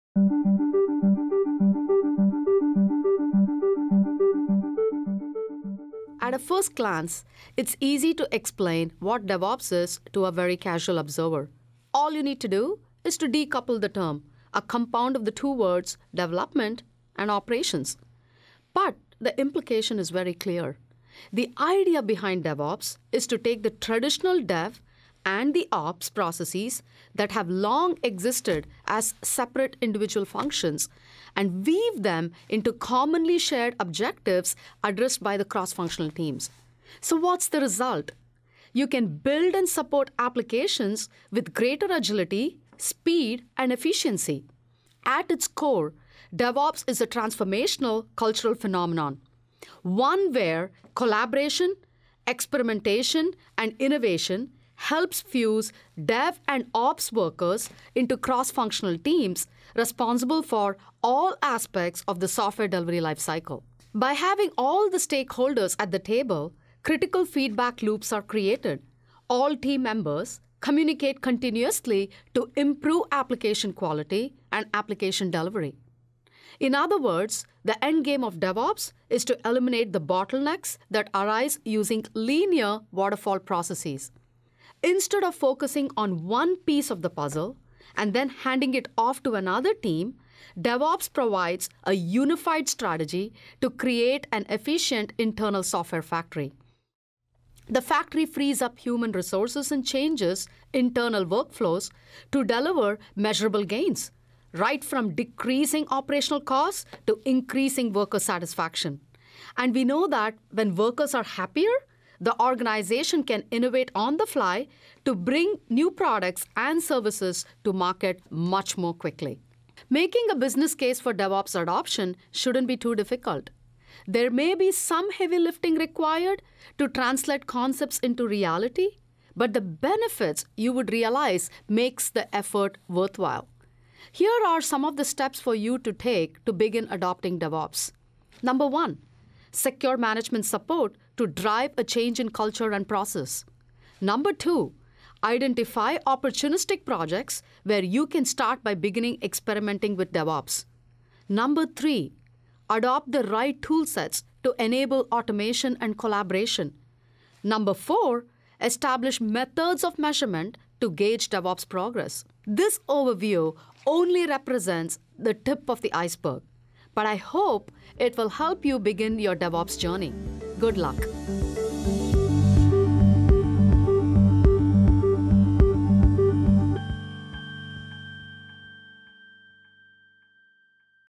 CA DevOps 101
DevOps takes traditional development and operations processes and weaves them into commonly shared objectives addressed by the cross functional teams. CA Technologies can help your organization get started with implementing DevOps practices in your business.